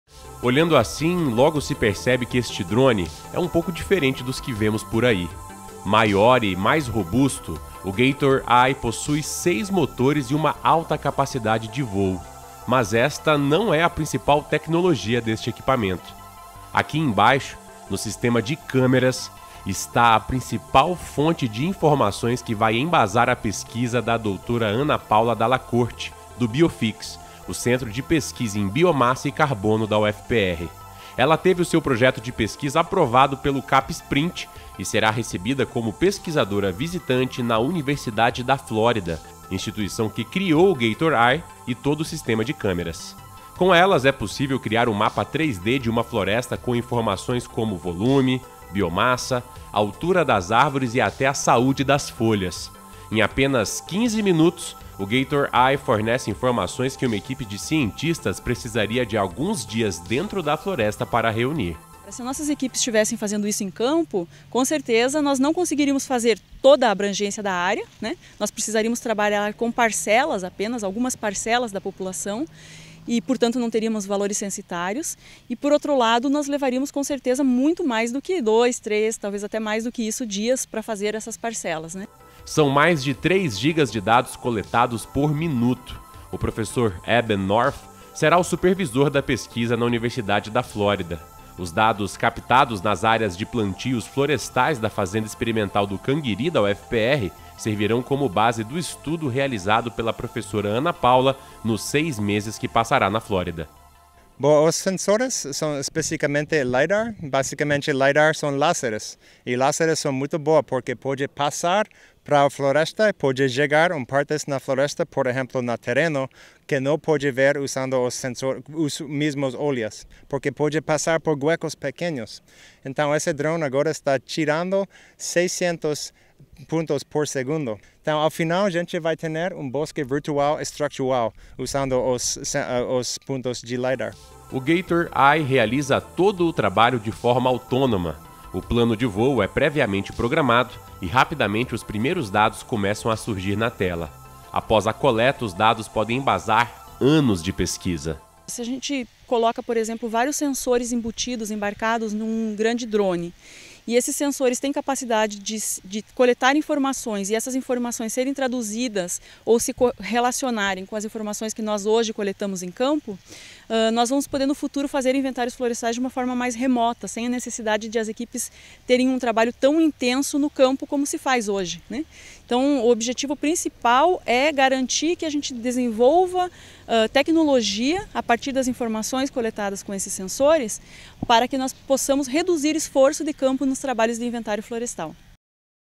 Informa | Pesquisadora da UFPR promove mapeamento florestal 3D
A tecnologia tem se tornado uma grande aliada dos pesquisadores no desenvolvimento de novos estudos. Esse é o caso de Ana Paula Dalla Corte, do Centro de Pesquisa em Biomassa e Carbono da UFPR, que teve seu projeto aprovado pelo CAPES-PrInt.

Com o auxilio de um drone, a pesquisadora pode criar um mapa florestal 3D e captar todas as informações necessárias sobre a vegetação local. Os dados coletados pelo drone servirão para embasar a pesquisa de Ana Paula, que irá atuar como pesquisadora visitante na Universidade da Flórida.